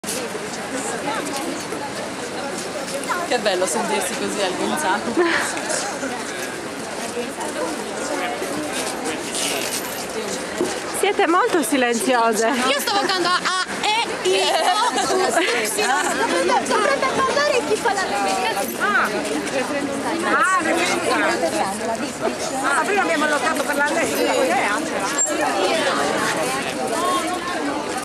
Flash mob "Ogni giorno è l'8 marzo - Diciamo insieme parole e di(s)senso" - video 6
Donne in Movimento in movimento: alla ricerca di luoghi di resistenza!

Donne in Movimento ritorna in piazza e promuove il Flash mob "Ogni giorno è l'8 marzo - Diciamo insieme parole e di(s)senso"!

L'appuntamento è per Domenica 6 marzo, alle 10:30, in Piazzale Trento a Cagliari. Componiamo parole: parole di senso e dissenso.